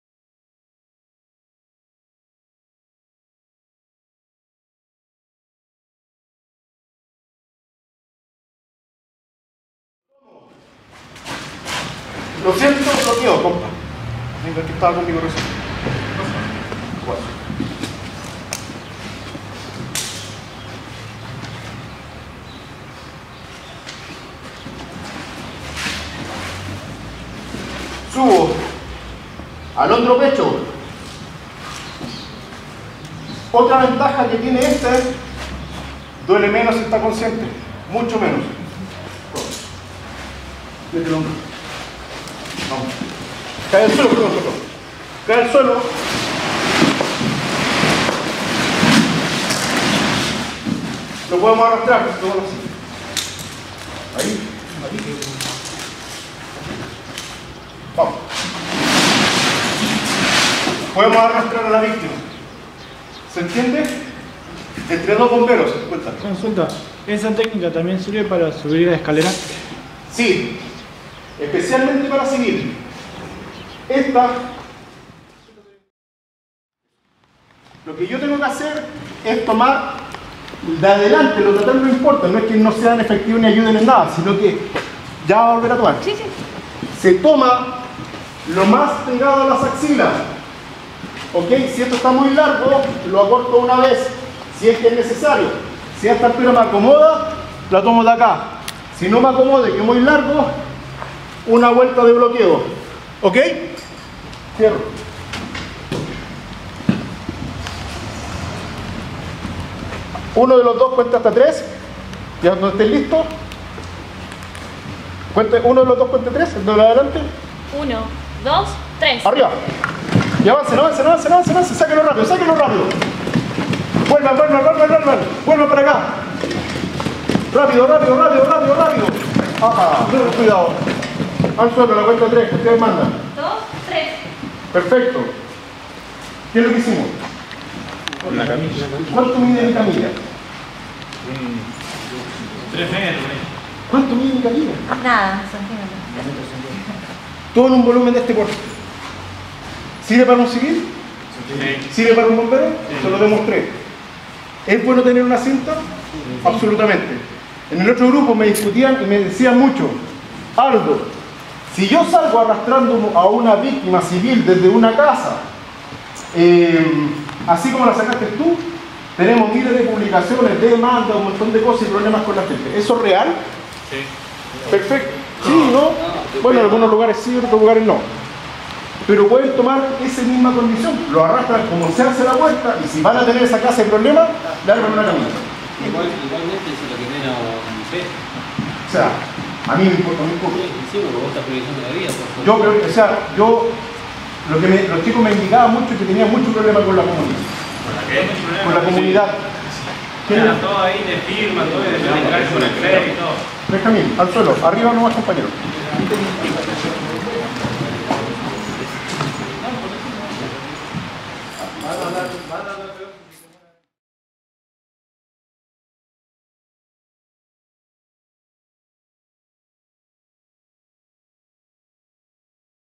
"CURSO RIT OPERACIONES" EN BOMBEROS DE TRELEW - 06 AL 10 DE OCTUBRE 2022
REALIZACIÓN DE LOS CURSOS "USO DE EQUIPOS DE RESPIRACIÓN AUTÓNOMA EN OPERACIONES DE BÚSQUEDA DE VÍCTIMAS & RIT OPERACIONES" JUNTO AL CUERPO DE BOMBEROS VOLUNTARIOS DE TRELEW, CHUBUT EN ARGENTINA - 06 AL 10 DE OCTUBRE 2022